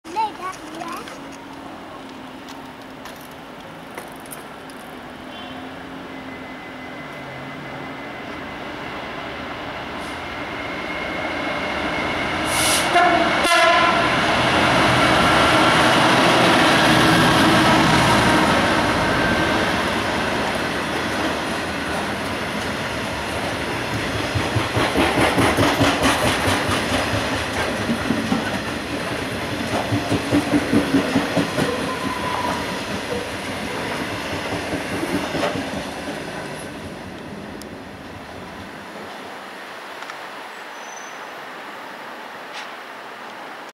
DBS Class 66 66106 at Denmark Hill on the 640Z 17/02/2016
DBS Class 66 66106 at Denmark Hill on the 640Z 1057 Cliffe Brett Marine to Neasden Engineers Sdgs 17/02/2016

66106  EWS DBS WBBT  TO